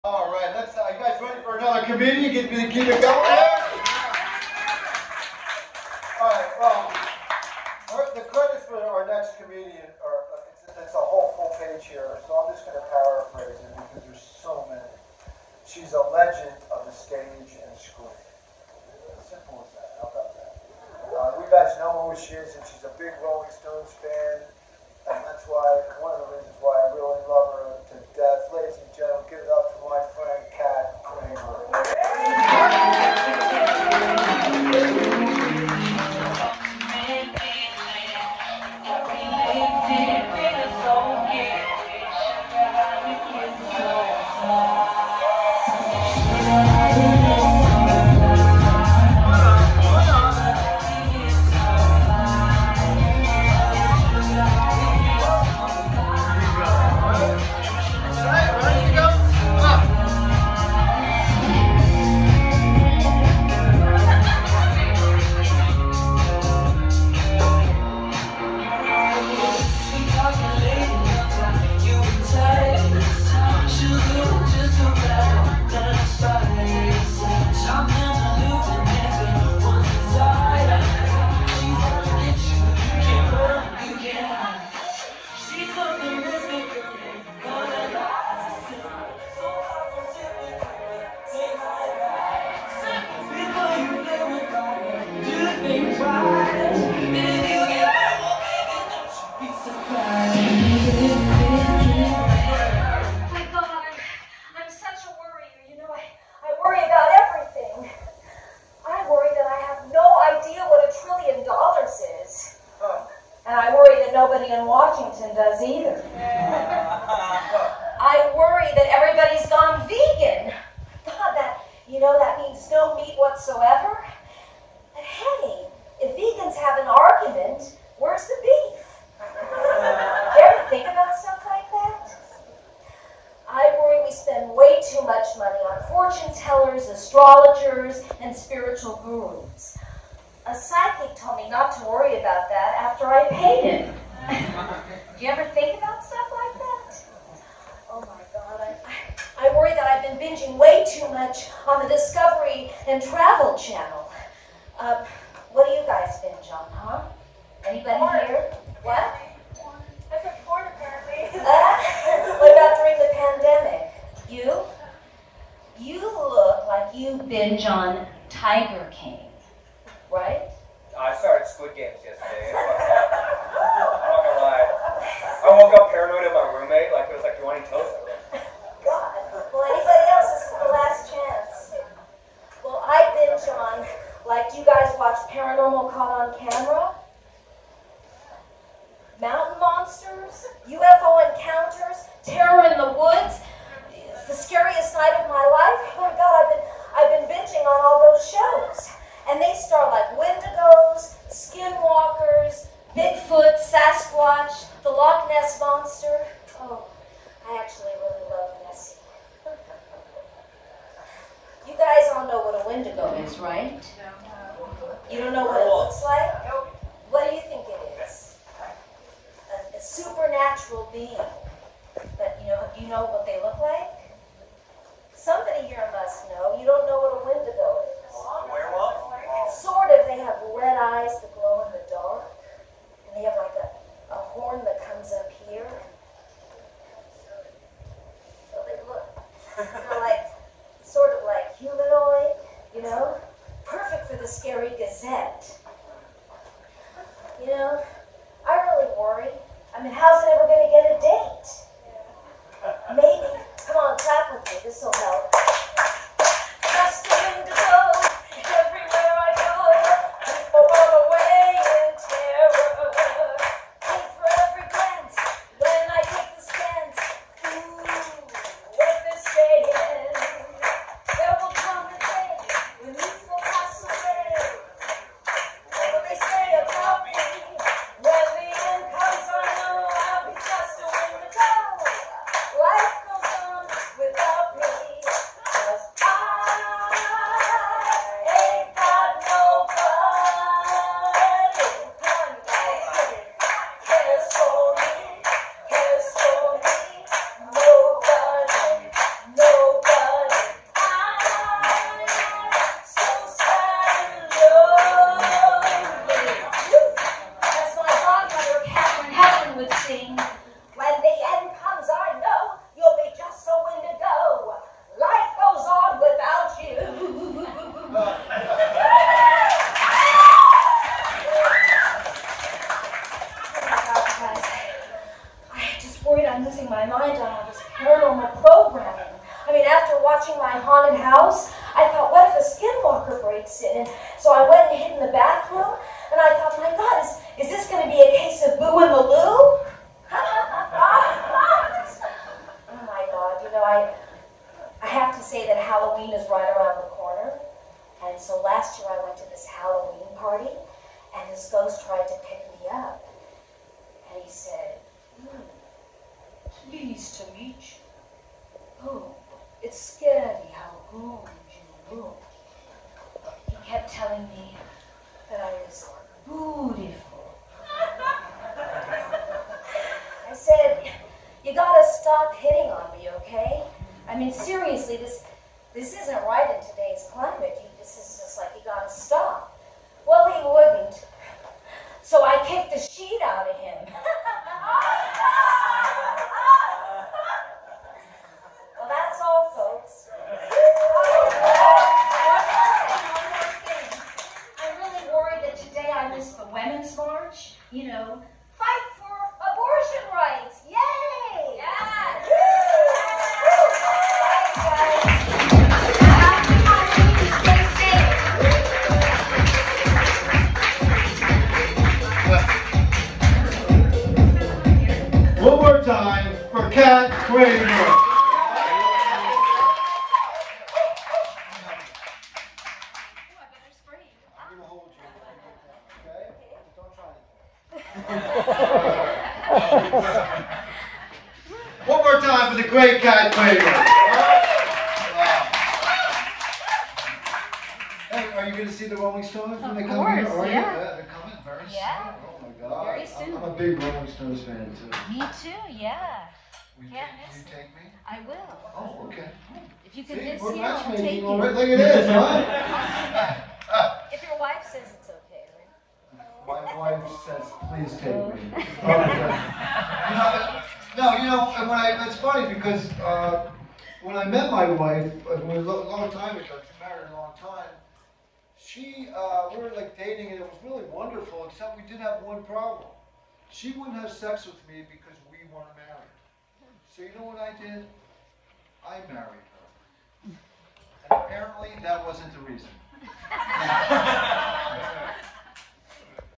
KAT KRAMER COMEDY- FELIX & FRIENDS- THE COMEDY CHATEAU - ( October 2nd, 2021)
Kat Kramer makes her "Felix & Friends" debut at The Comedy Chateau. Her brand of "Kat Kramer Comedy" is original, yet also  a throwback to classic comedians of yesteryear.

Her Halloween themed topics ranged from worrying about excessively binging on paranormal programming, to windegos, skinwalkers, and ghosts.

Kat is inspired by Lily Tomlin, socially-conscious satirical comedy, plus opens with a bit, reminiscent of an early Phyllis Diller routine. KNK Productions,Inc.